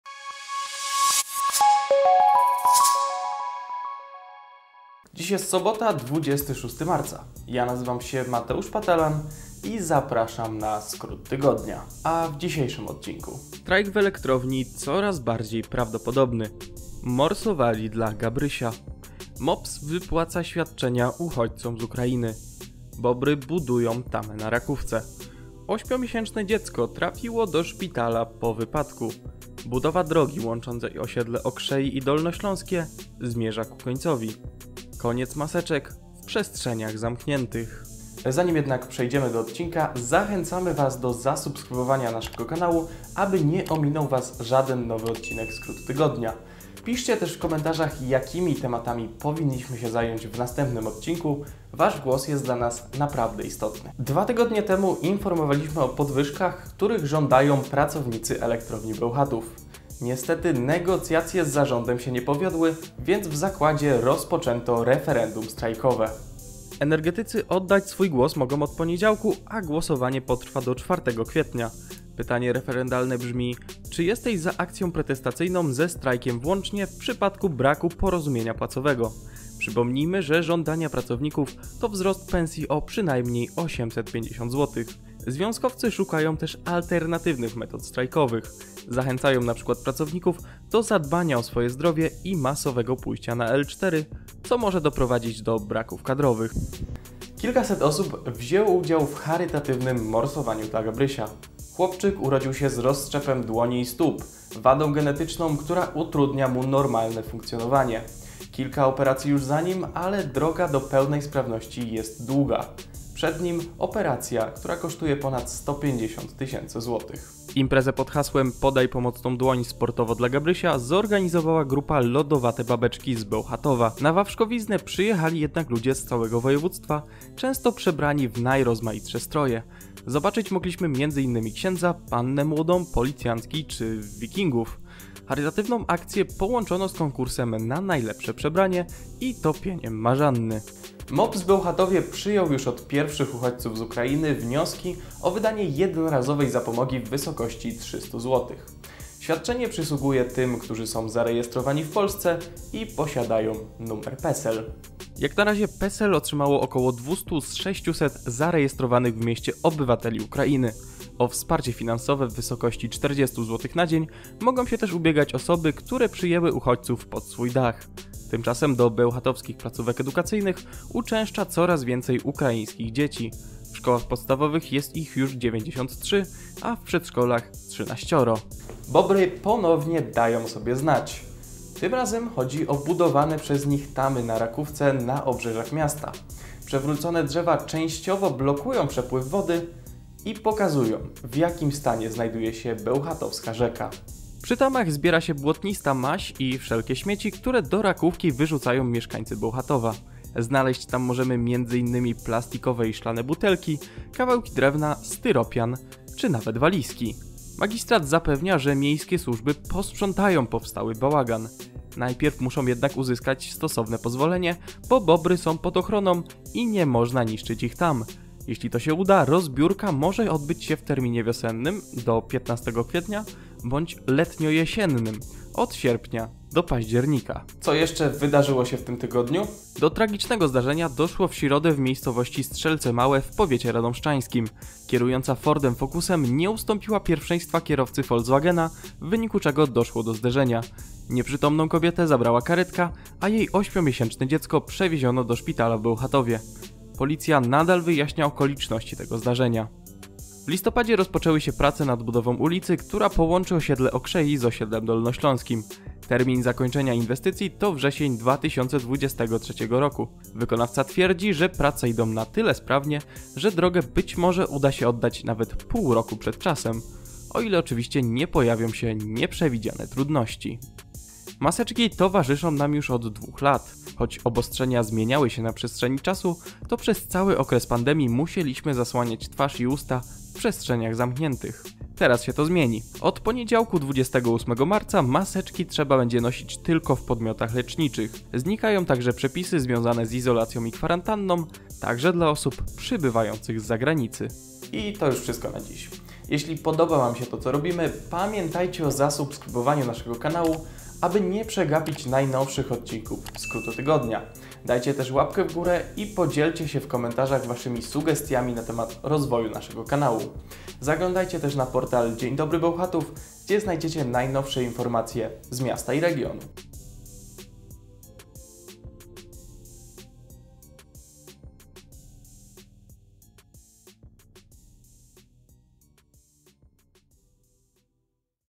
Strajk w Elektrowni? | Morsowali dla Gabrysia | Bobry budują tamę na Rakówce – Skrót Tygodnia #3
W dzisiejszym wydaniu Skrótu Tygodnia:
0:54 - Strajk w Elektrowni Bełchatów coraz bliżej?
1:36 - Morsowali dla Gabrysia
2:20 - Zapomogi dla uchodźców z Ukrainy
2:58 - Bobry budują tamę na Rakówce
3:47 - 8-miesięczne dziecko trafiło do szpitala po wypadku
4:09 - Budowa drogi łączącej os. Okrzei i Dolnośląskie zakończy się wcześniej?
4:30 - Od poniedziałku pożegnamy maseczki